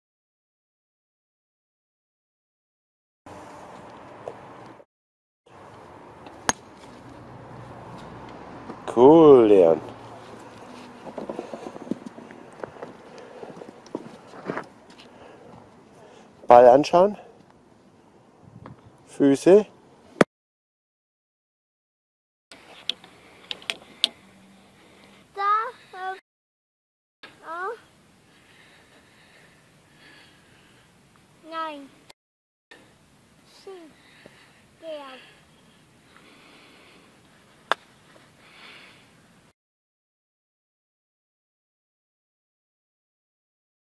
Leon 3 Jahre alt spielt Golf und liebt sein U.S. Golf Kids Bag
Anfangs haben wir ihm sein eigenes Spielzeug mitgenommen, wenn wir auf die Range gingen, aber dann wollte er auch Golf spielen und sein Spielzeug war uninteressant, also haben wir ihm ein U.S. Kids Golf Bag besorgt. Er hat riesen Freude daran. Mittlerweile haben wir ihn beim GC Fürth im Bambinigolf angemeldet dann  kann er es richtig lernen, die haben eine tolle Kinder- und Jugendarbeit. Da ist er auch unter gleichaltrigen.(Bambinis 3-6Jahre)

3 year old boy playing Golf